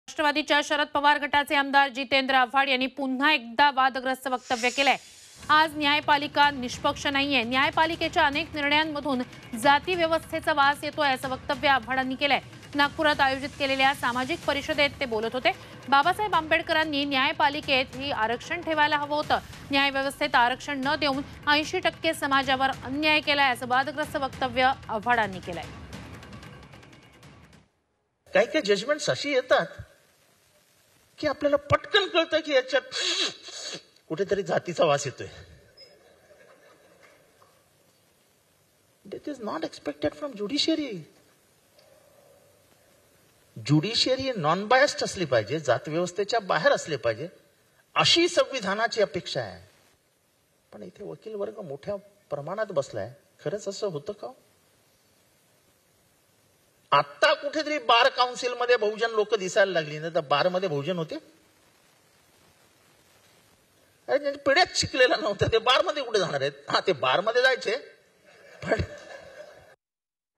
Jitendra Avhad Contraversial Statement | आव्हाड यांचे आरक्षणाच्या मुद्द्यावरुन वादग्रस्त विधान
राष्ट्रवादीच्या शरद पवार गटाचे आमदार जितेंद्र आव्हाडांनी पुन्हा एकदा वादग्रस्त विधान केलंय.. आज न्यायपालिका निष्पक्ष नाही... न्यायपालिकेच्या अनेक निर्णयांमधून जातीव्यवस्थेचा वास येतो असं विधान आव्हाडांनी केलंय.. नागपुरात आयोजित केलेल्या सामाजिक परिषदेत आव्हाड बोलत होते.. बाबासाहेब आंबेडकरांनी न्यायपालिकेमध्ये ही आरक्षण ठेवायला हवं होतं. न्यायव्यवस्थेमध्ये आरक्षण न देऊन 80 टक्के समाजावर अन्याय केला असं वादग्रस्त विधान आव्हाडांनी केलंय.

NCP Sharad Pawar MLA Jitendra Awhad has once again made a controversial statement.. Judiciary is not fair today... Awhad has made the statement that there is a smell of caste system in many decisions of the judiciary.. Awhad was speaking in a social conference organized in Nagpur.. Babasaheb Ambedkar should keep this reservation in the judiciary. was Awhad made a controversial statement that 80 percent of the society was wronged by not giving reservation in the judicial system.